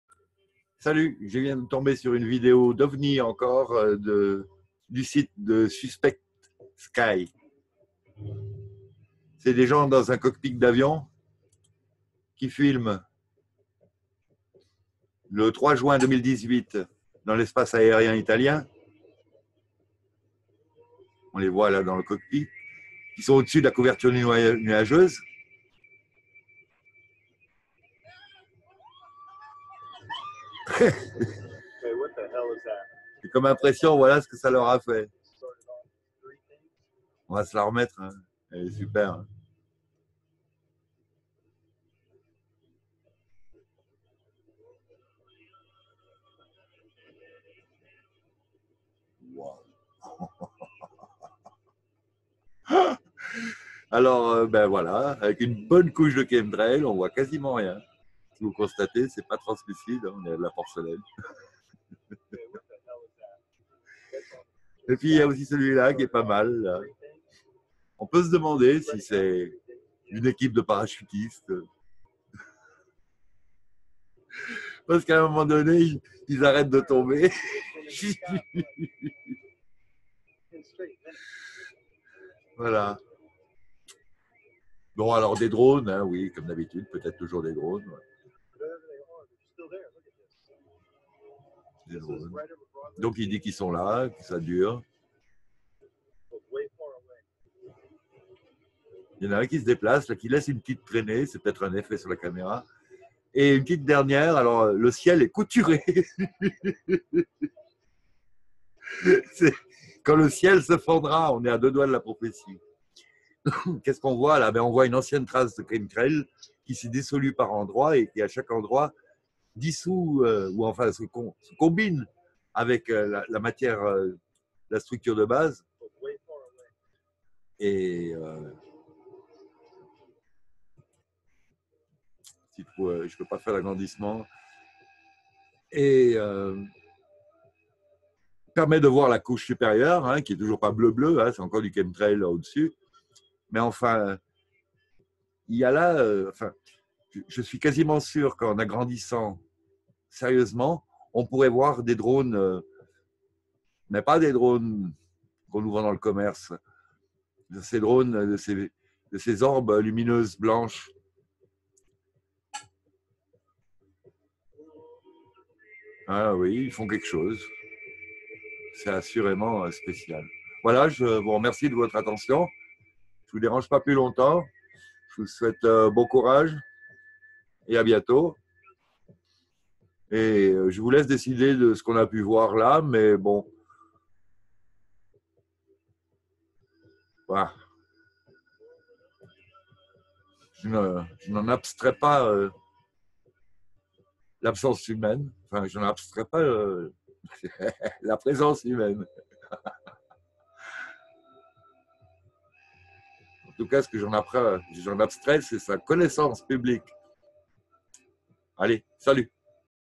UFOVNIPAN #14 LUMIERES ETRANGES DANS LE CIEL
Filmé depuis le cockpit, une SPHERE LUMINEUSE s'élève au dessus des nuages.
Dans le ciel américain, un groupe de lumières arrètent de chuter pour s'égailler dans l'horizon.
Un ex-CHEMTRAIL signe sa trace de l'effet particulier et à intervalles réguliers, de quelque étrange système qu'il aurait pu semer dans NOTRE ATMOSPHERE.
VO: UFO Rising Through Clouds Captured From Airplane [SIGHTINGS]